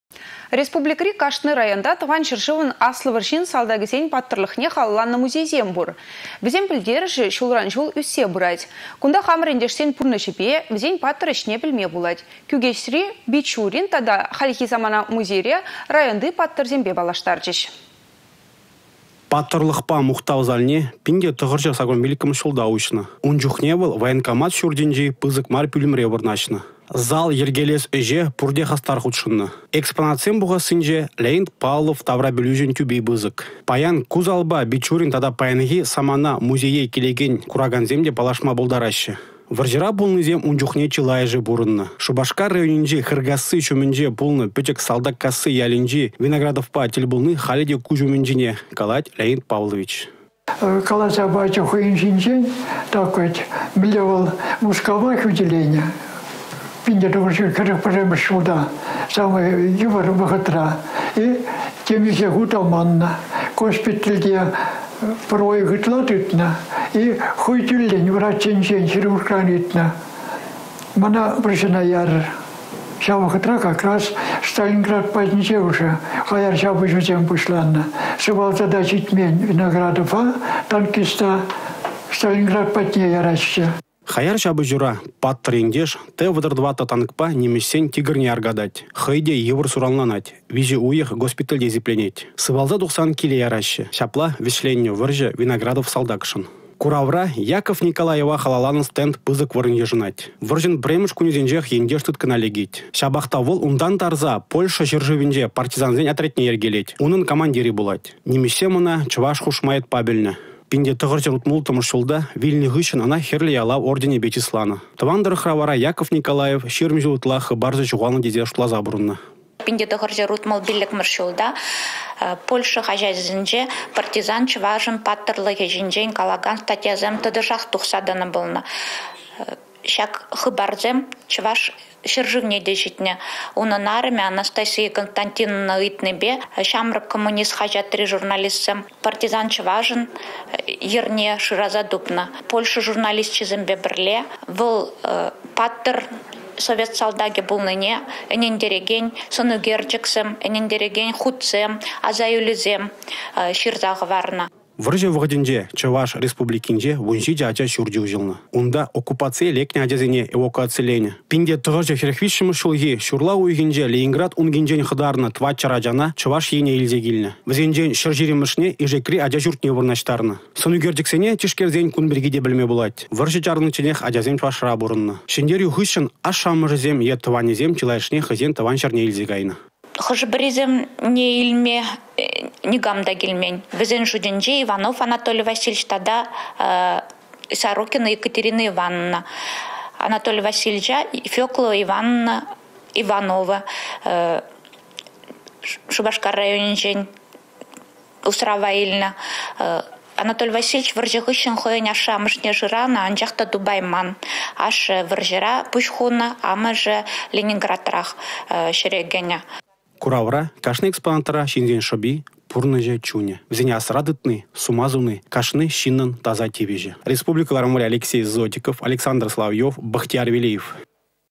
Паттăрлăхпа мухтав залĕ
Республикăри кашни районта Тăван Çĕршывăн Аслă вăрçин салтакĕсен паттăрлăхне халалланă музейсем пур. Вĕсен пĕлтерĕшĕ çулсерен ÿссе пырать. Кунта хамăр ентешсен пурнăçĕпе, вĕсен паттăр ĕçне пĕлме пулать. Кÿкеçри «Бичурин тата хальхи самана» музейре районти паттăрсемпе паллаштарчĕç.